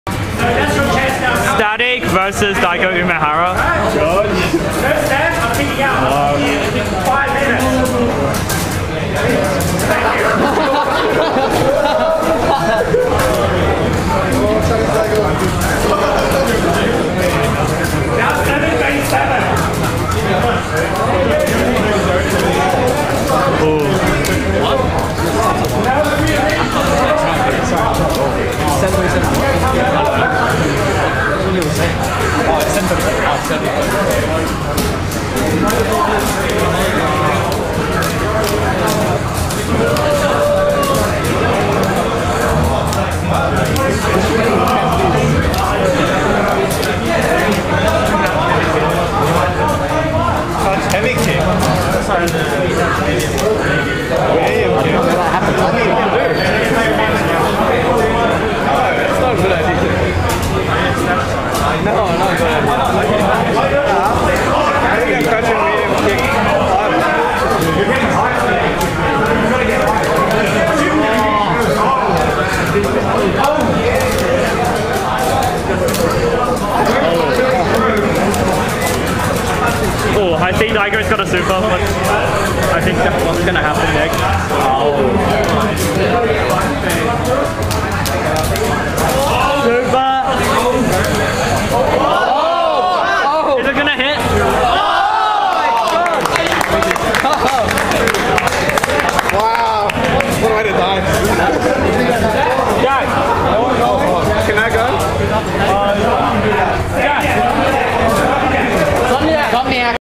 Pre EVO APAC Daigo Session: Static
We held a casual/money match session for Daigo the night before EVO APAC

Static v Daigo Umehara

28 May 2010 @ Good Games Sydney

Filmed using a Kogan 1080p HD Camcorder